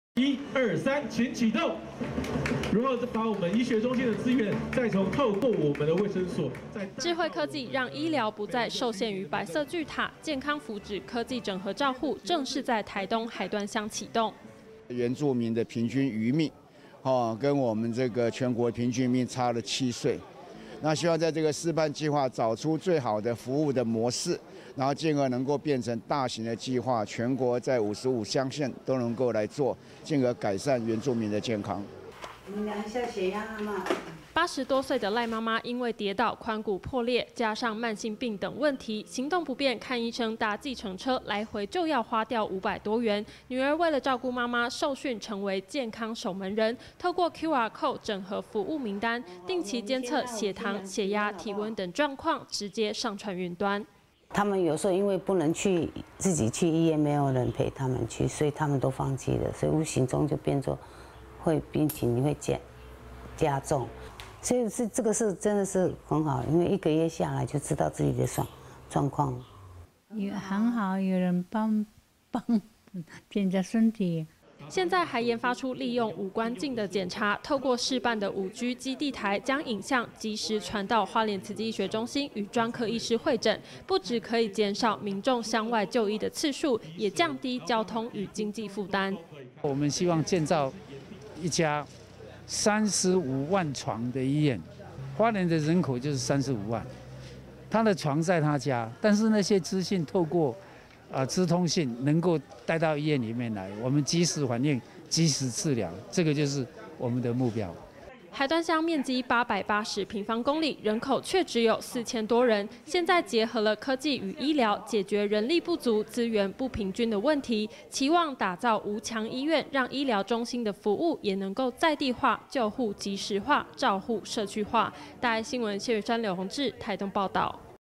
海端試辦遠距看診 打造無牆醫院
台東海端鄉幅員廣闊、人口又少，距離最近的花蓮慈濟醫學中心，有一百多公里的距離，為了讓醫學中心的服務能夠在地化，今天在台東縣海端鄉衛生所舉辦了「健康福祉科技整合照護」啟動記者會，台東縣政府、花蓮慈濟醫學中心與國內電信業者合作，將利用5G網路大寬頻、低遲延的特性，讓海端鄉試辦遠距看診，打造無牆醫院。

1、2、3，請啟動。

智慧科技讓醫療不再受限於白色巨塔，健康福祉科技整合照護正式在台東海端鄉啟動。

衛福部長 陳時中：「原住民的平均餘命，跟我們這個全國平均餘命差了7歲，希望在這個試辦計畫，找出最好的服務的模式，進而能夠變成大型的計畫，全國在55(原鄉)鄉縣都能夠來做，進而改善原住民的健康。」

八十多歲的賴媽媽因為跌倒髖骨破裂，加上慢性病等問題，行動不便看醫生搭醫搭計程車來回就要花掉五百多元，女兒為了照顧媽媽受訓成為健康守門人，透過QR-CODE整合服務名單，定期監測血糖、血壓體溫等狀況直接上傳雲端。

健康守門人 賴美玲：「他們有時候因為不能去，自己去醫院 沒有人陪他們去，所以他們都放棄了，所以無形中就變作 會病情會加重，所以是這個是真的很好，因為一個月下來就知道自己的狀況。」

部落長者 賴林源妹：「很好啊 有人幫我們檢查身體。」

現在還研發出利用五官鏡的檢查，透過試辦的5G基地台，將影像即時傳到花蓮慈濟醫學中心與專科醫師會診，不只可以減少民眾鄉外就醫的次數，也降低交通跟經濟負擔。

慈濟醫療志業執行長 林俊龍：「我們希望建造一家 35萬床的醫院，花蓮的人口就是35萬，他的床在他家 但是那些資訊，透過資通訊能夠帶到醫院裡面來，我們即時反應 及時治療，這個就是我們的目標。」

海端鄉面積880 平方公里，人口卻只有4千多人，現在結合了科技與醫療，解決人力不足資源不平均的問題，期望打造無牆醫院，讓醫療中心的服務能在地化、救護即時化、照護社區化。

謝昀珊 劉泓志 台東報導